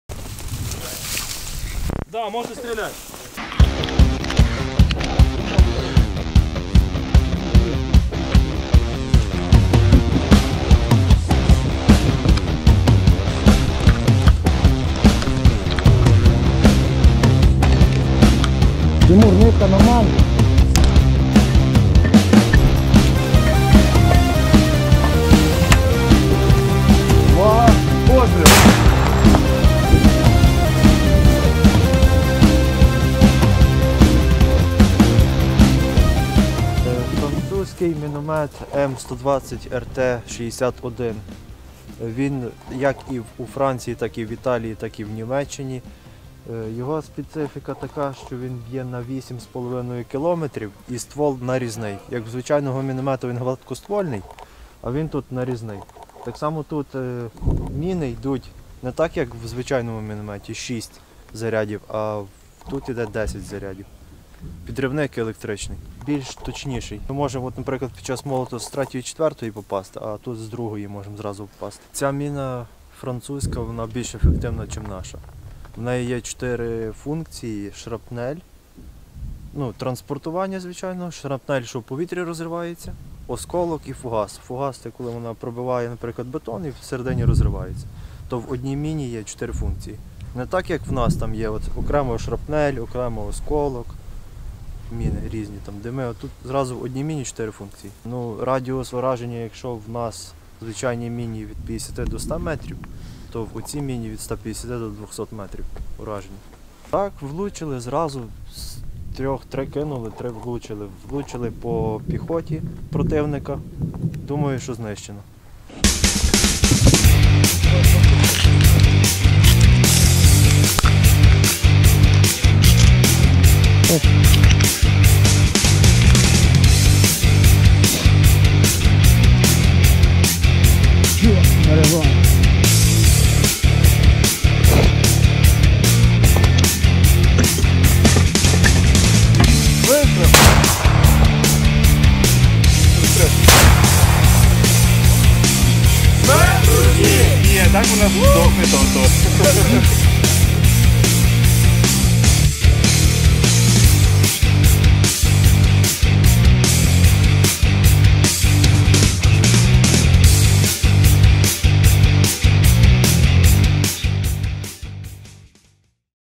Application and assessment of French 120 mm mortars from the legendary 93 Kholodny Yar brigade
, whose soldiers defended Bakhmut and Soledar

Застосування та оцінка французьких 120мм мінометів від легендарної 93 бригади Холодний Яр, воїни якої обороняли Бахмут та Соледар